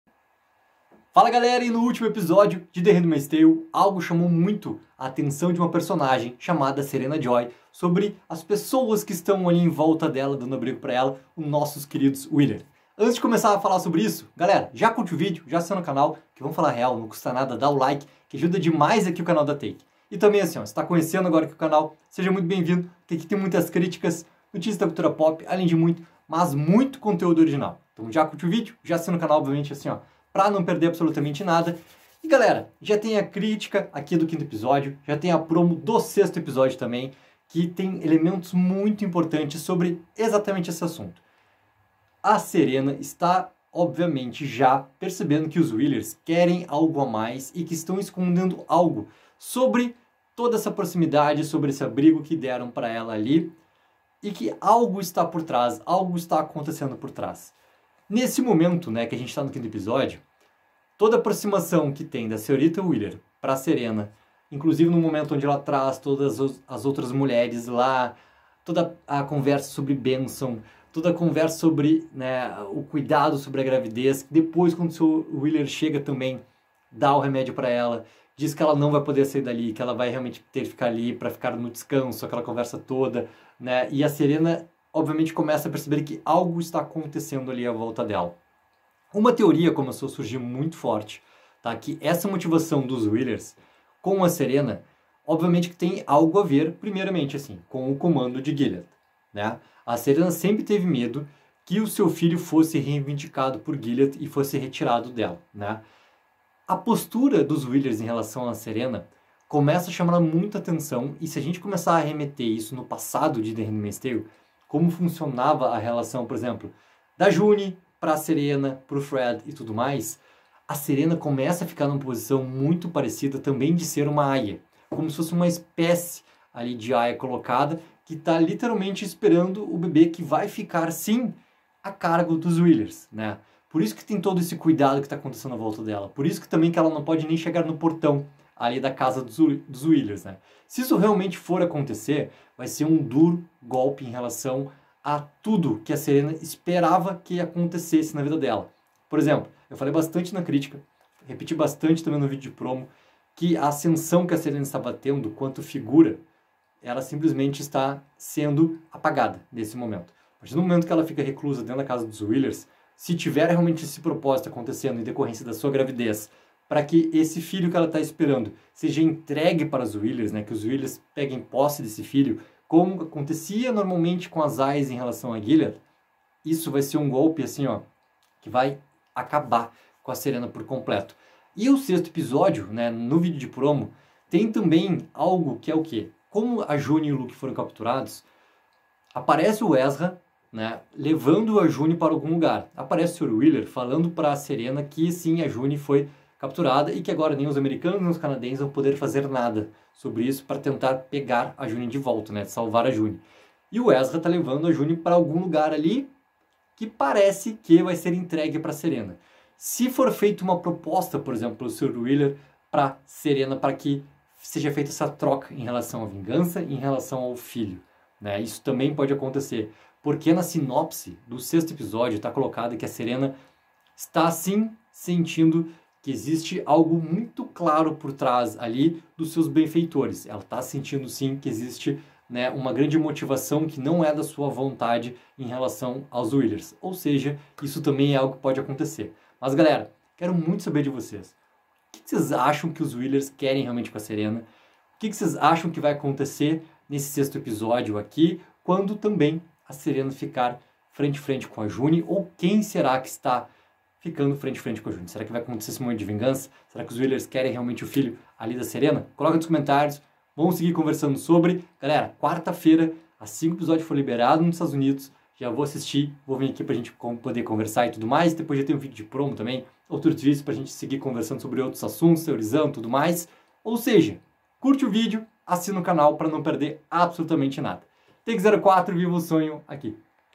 SERENA VAI PERDER SEU FILHO PARA OS WHEELERS?
Na 5ª temporada, June enfrenta consequências por matar o Comandante Waterford enquanto luta para redefinir sua identidade e propósito. A viúva Serena tenta aumentar seu perfil em Toronto enquanto a influência de Gilead se infiltra no Canadá. O comandante Lawrence trabalha com tia Lydia enquanto tenta reformar Gilead e subir no poder. June, Luke e Moira lutam contra Gilead à distância enquanto continuam sua missão de salvar e se reunir com Hannah. Confira agora e não esqueça de curtir o vídeo e assinar o canaaaaaaaaaaaaaaaaaaaaaal!
Quer mais conteúdo exclusivo? Então nos siga no instagram: @take04news @lukeschatzmann